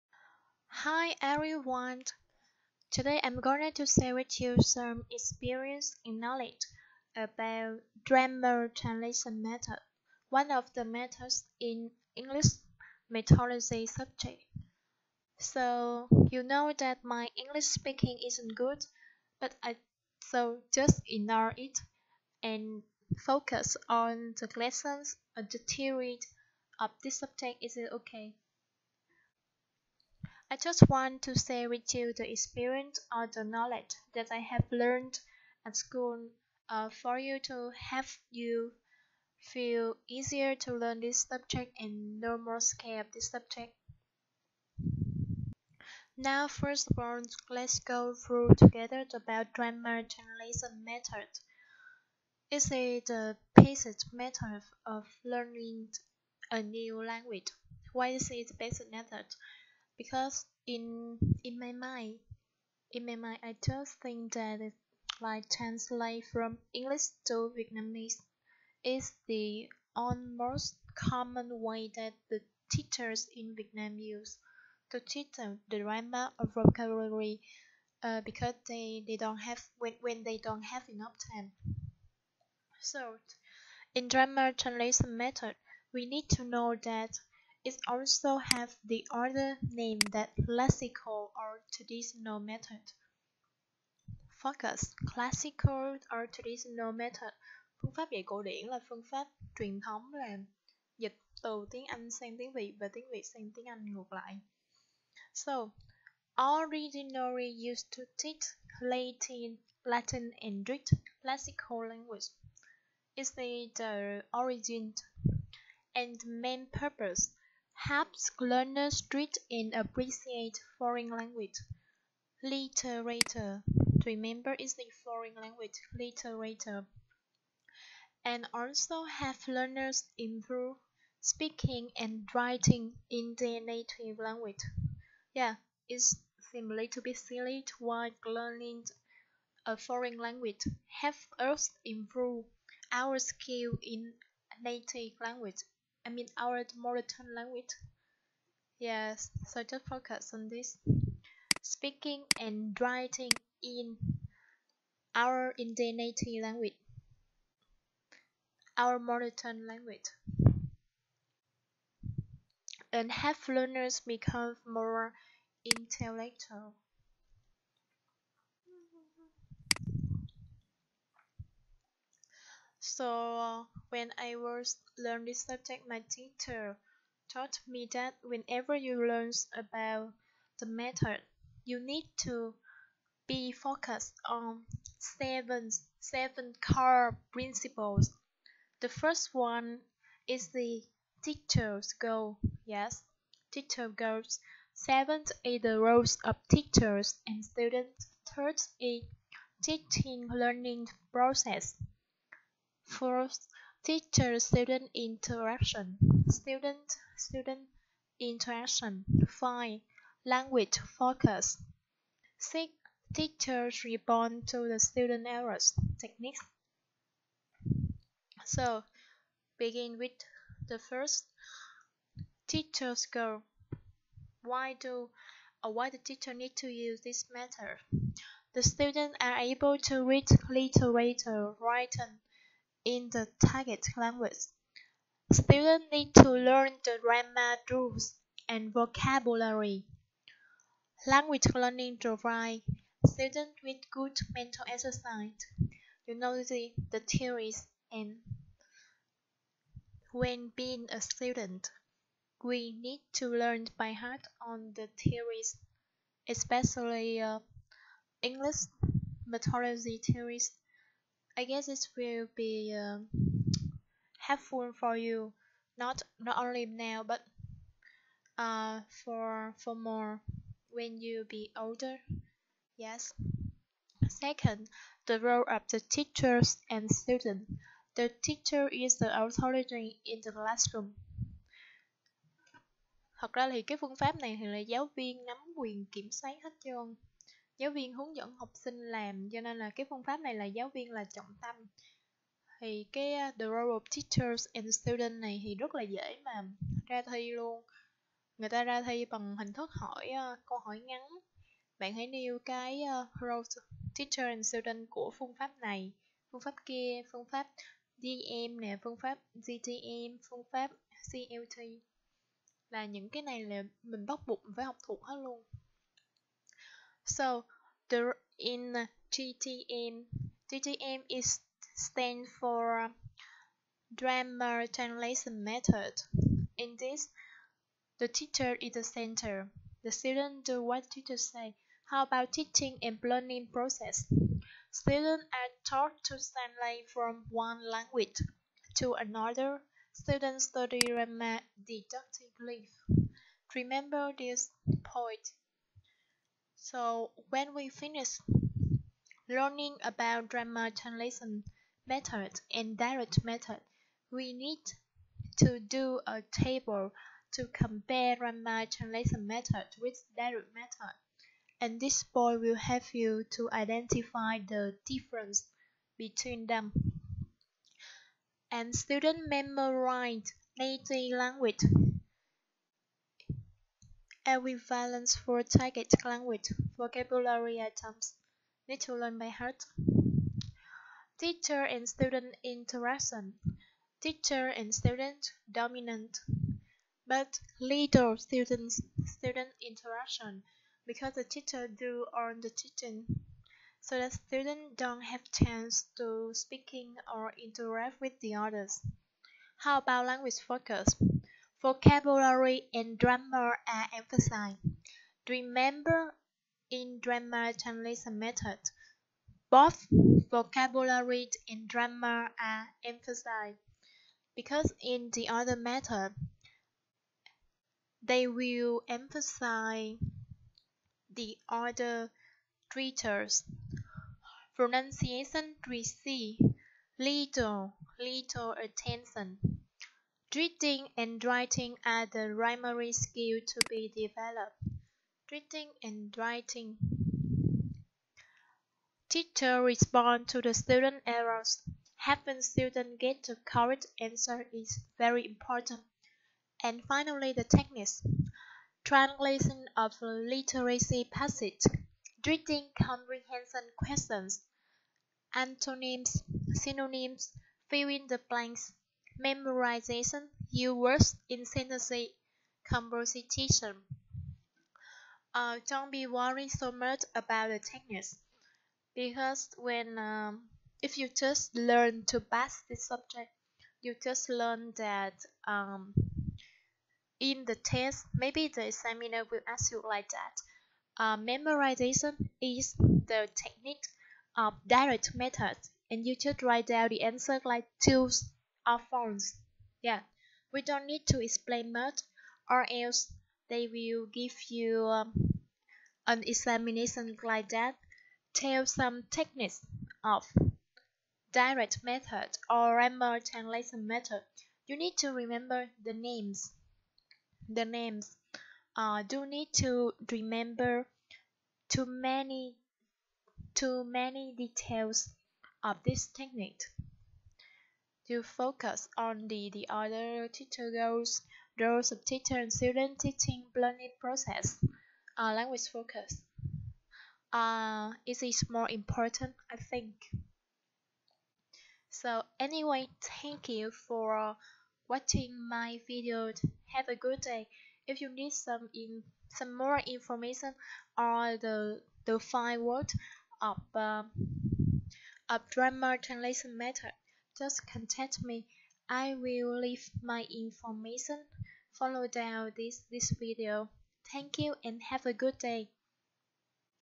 Grammar Translation Method
This is some information that I have noted down when I studied at school.
I know that my English speaking is not good, so just ignore it and focus on the information that I really want to share with you! I hope you will find this subject no more difficult.